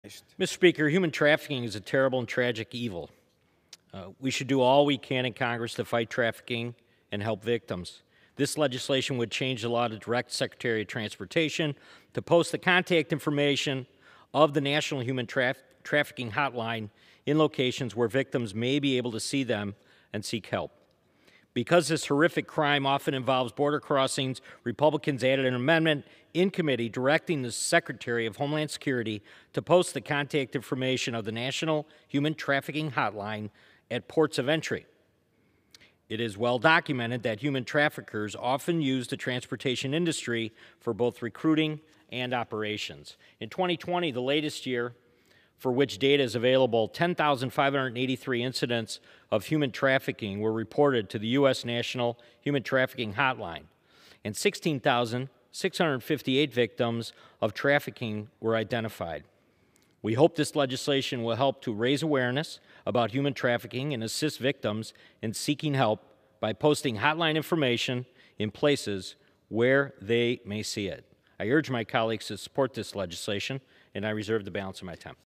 ‘This Horrific Crime Often Involves Border Crossings’: GOP Lawmaker Condemns Human Trafficking
During House Floor remarks last month, Rep. Scott Fitzgerald (R-WI) spoke about human trafficking.